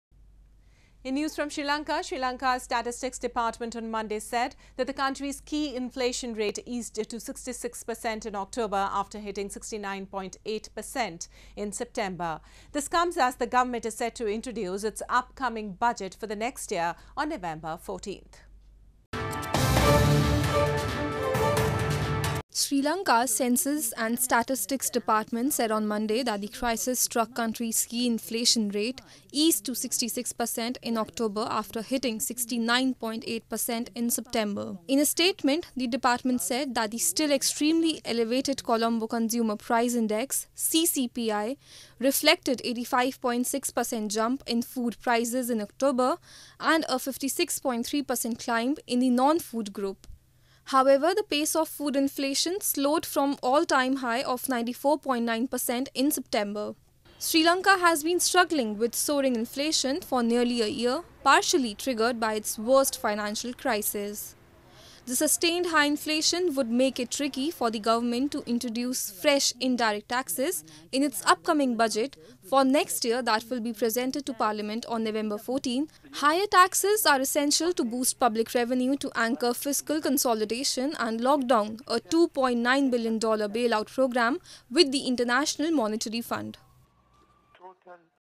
Sri Lanka's key inflation rate eases to 66% in October
Sri Lanka's statistics department on Monday said that the country's key inflation rate eased to 66% in October after hitting 69.8% in September. This comes as the government is set to introduce its upcoming budget for the next year on November 14.